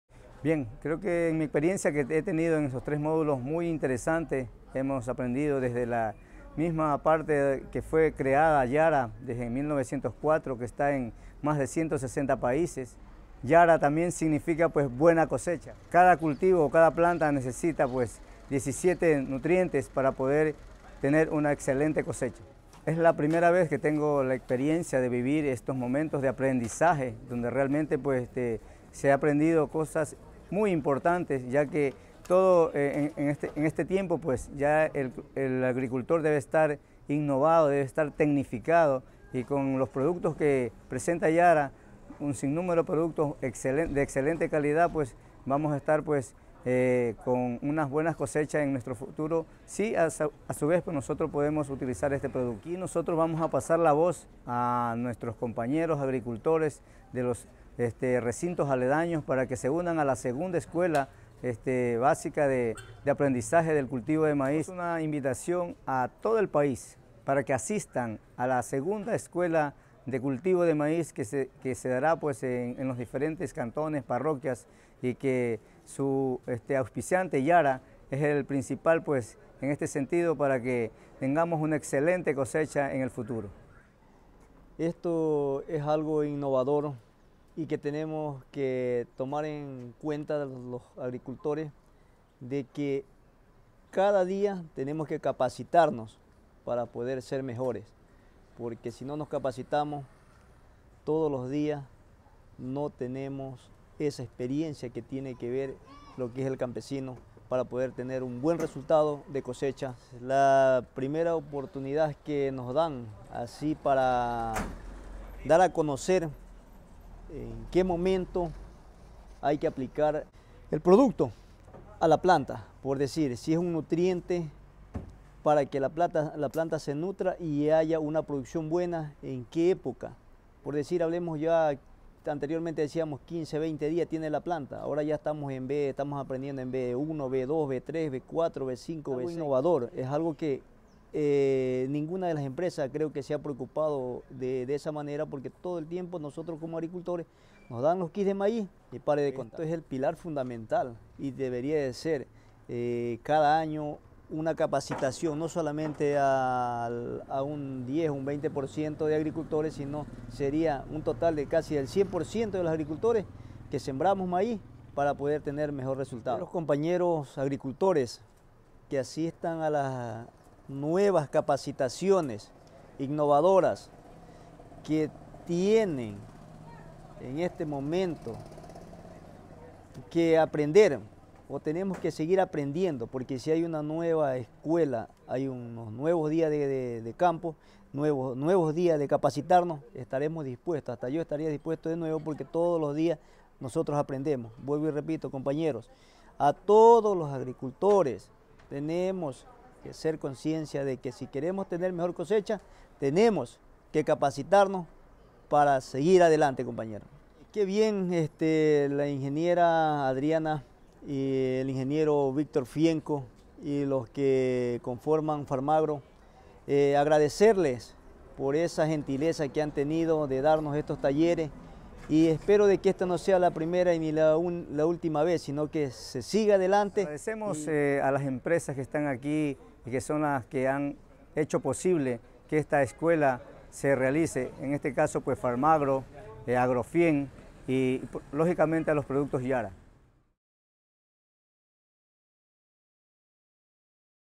EC- Testimonio Resultados Escuelas Maiz
Conoce los testimonios de los agricultores participantes en la escuela de cultivos de Yara y como estas escuelas les ayuda a ser mas productivos con sus cultivos